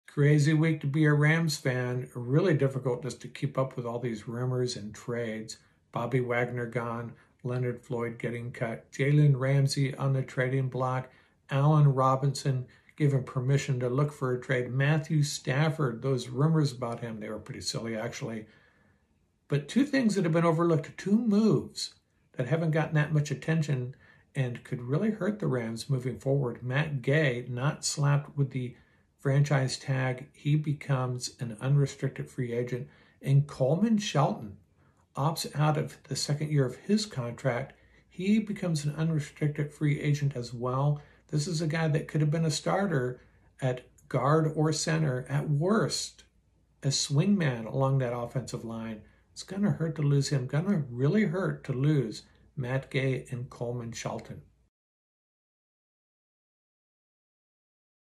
Don’t Overlook the Impact of Potentially Losing Gay and Shelton
Losing Leonard Floyd, Jalen Ramsey, Allen Robinson, and Bobby Wagner will obviously have an impact on our Los Angeles Rams. But don't overlook the impact of potentially losing interior offensive lineman Coleman Shelton and kicker Matt Gay.